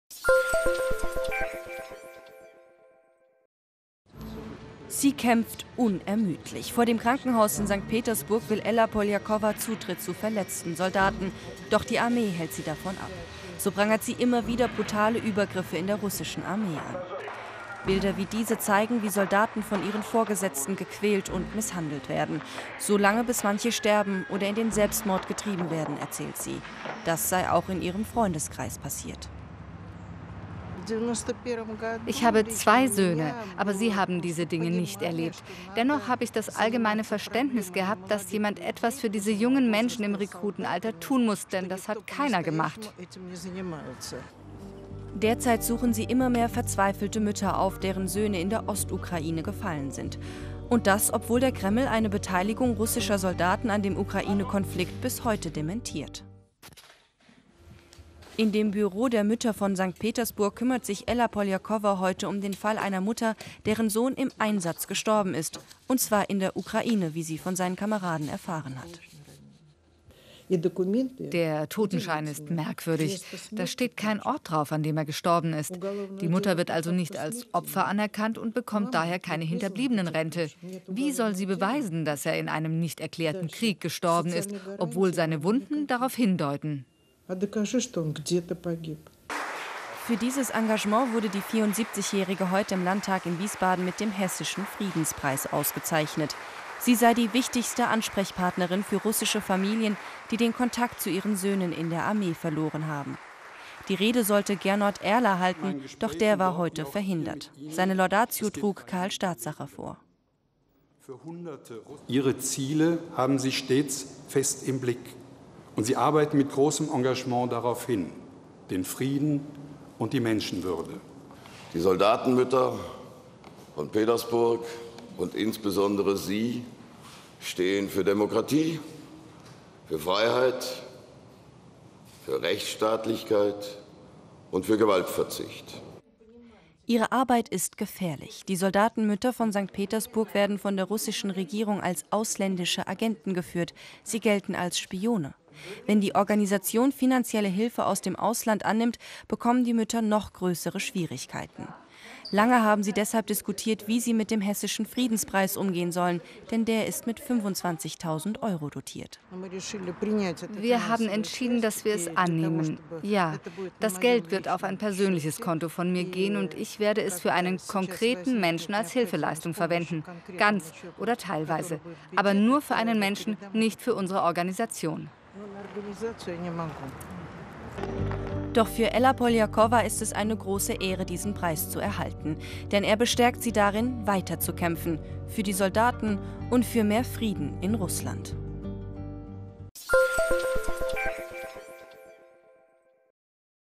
Hessischer Friedenspreis an Ella Polyakova in Wiesbaden verliehen
kämpft gegen Missstände in der russischen Armee. Die Leiterin Ella Polyakova, der Organisation "Soldatenmütter von St. Petersburg" kämpft seit Jahrzehnten für die Rechte Wehrpflichtiger in der russischen Armee.Sie setze in konfliktreichen Zeiten ein mutiges Zeichen für Frieden und Demokratie, sagte Ministerpräsident Volker Bouffier (CDU) am Freitag in Wiesbaden bei der Verleihung des Friedenspreies. Er ist mit 25.000 Euro dotiert und wird seit 1994 im Landtag verliehen.